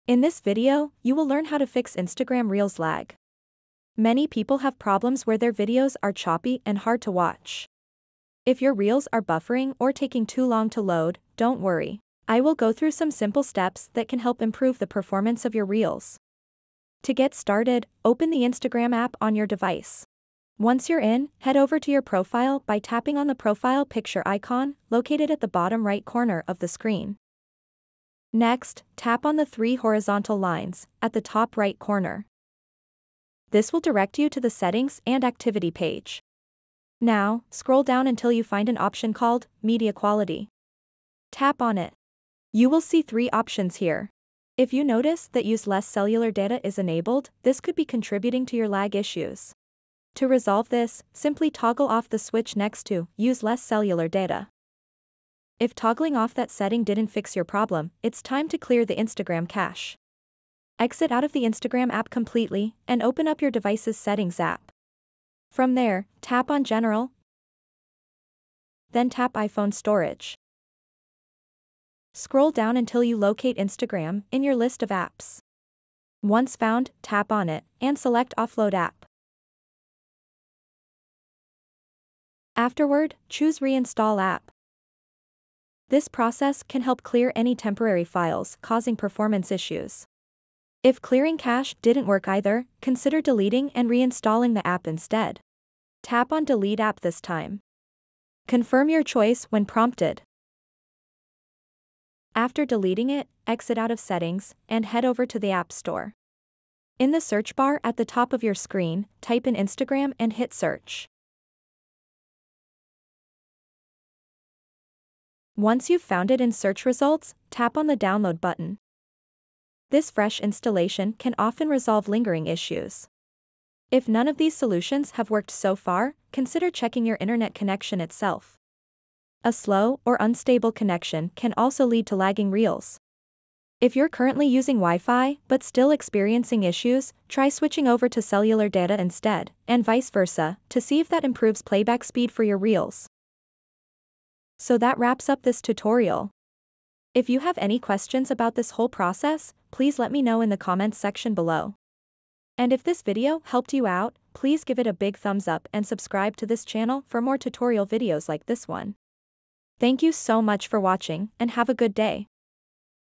How To Fix Instagram Reels Lag
In this video, you will learn How To Fix Instagram Reels Lag. Many people experience lag when trying to watch their favorite videos. Sometimes, the reels take a long time to load or get stuck completely. This can be very frustrating, especially if you want to enjoy fun content quickly. So, we will share some helpful tips on how to fix these issues.